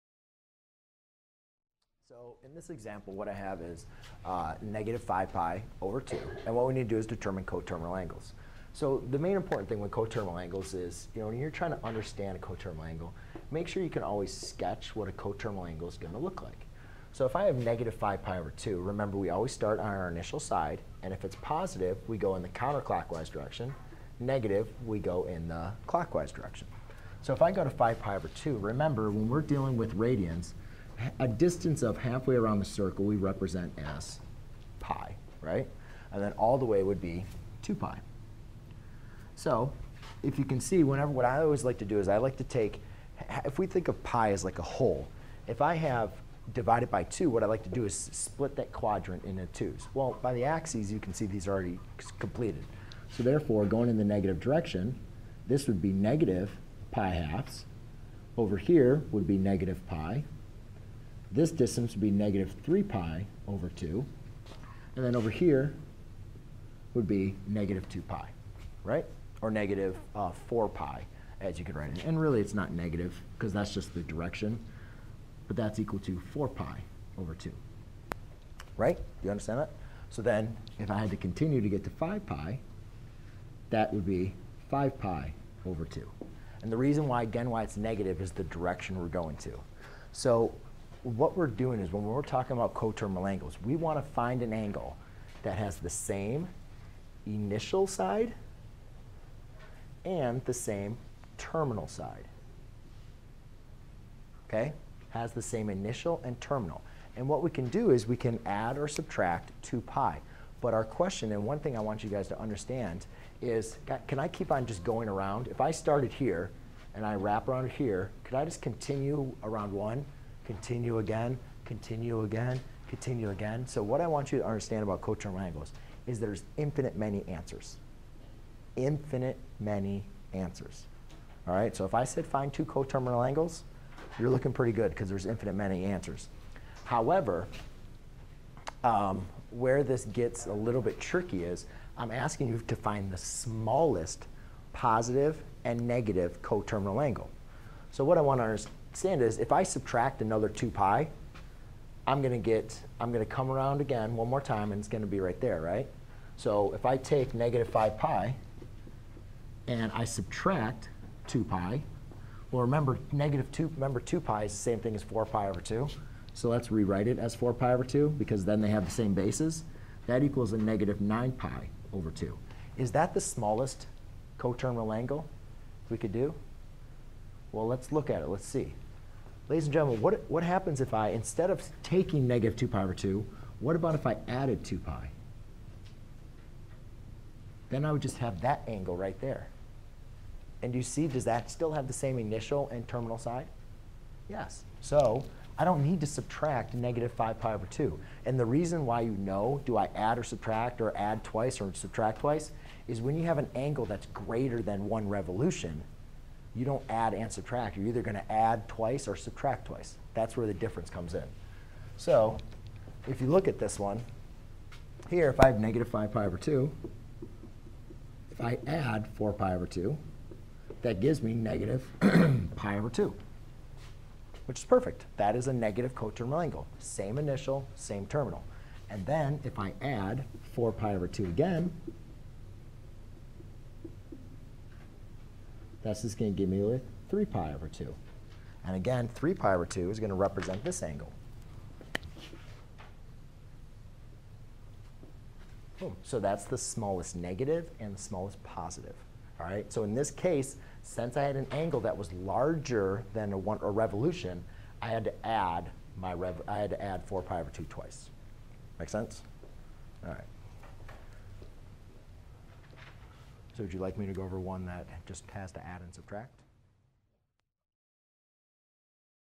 Determining the smallest positive and negative coterminal angles
👉 Learn the basics of co-terminal angles. An angle is a figure formed by two rays that have a common endpoint. The two rays are called the sides of the angle while the common endpoint is called the vertex of the angle. We measure angles starting from the positive x-axis, i.e. the initial side of an angle measure is usually the positive x-axis. The angle measured in the anti-clockwise direction is called a positive angle while a negative angle is measured in the clockwise direction.

Two or more angles are said to be co-terminal when they have the same initial and terminal sides. Coterminal angles are found by adding/subtracting 360 degrees (for degree angle measure) or 2pi (for radian angle measure) to/from the given angle.